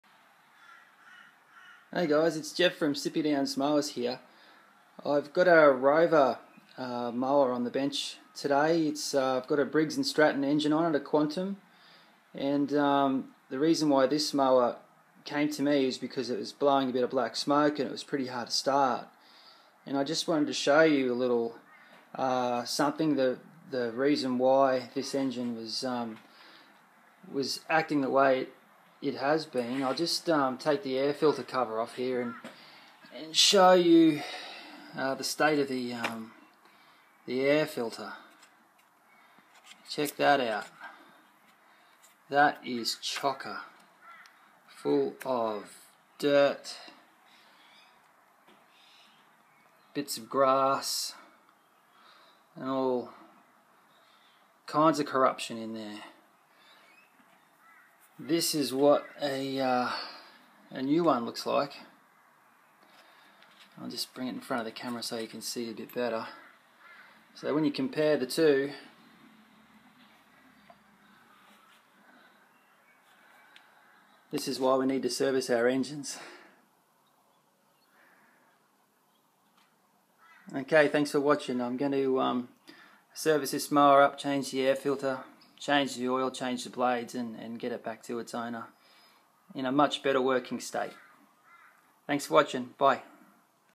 Hard to start rover mower
The blocked air filter on this machine made it hard to start, and when it did start it blew a lot of black smoke.  For more repair vids, check out the